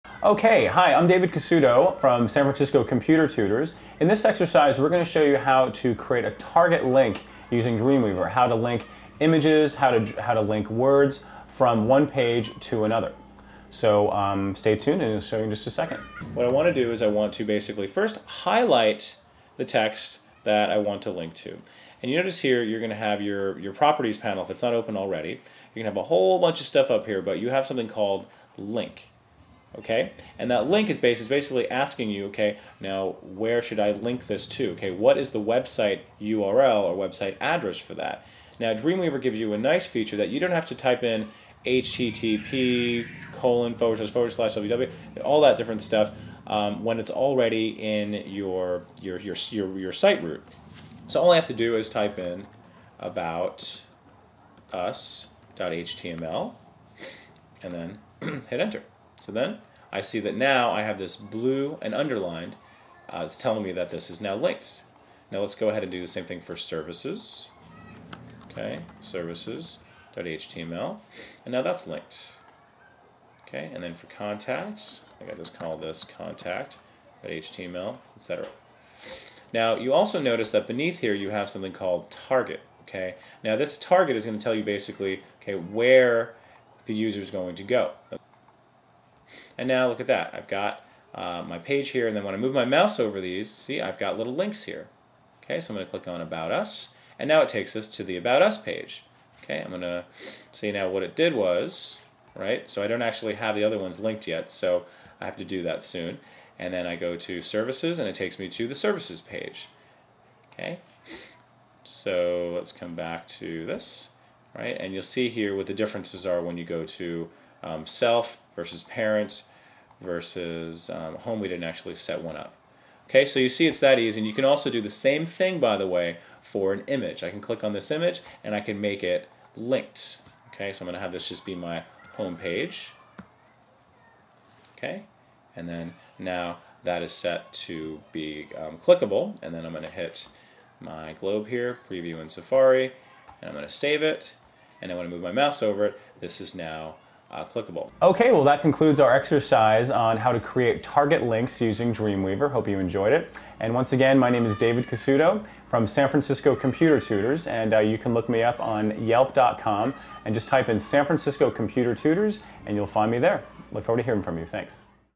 Dreamweaver Tutorial : How to Add a Target Link to a Web Page With Dreamweaver
To add a target link to a Web page with Dreamweaver software, highlight the item to be linked, click on the "Link" button on the properties panel and type in the desired URL. Link text and images to other Web pages with instructions from a certified Dreamweaver tutor in this free video on Web design.

Expert: Dave Casuto
Bio: Dave Casuto is a certified Dreamweaver tutor working in San Francisco, Calif.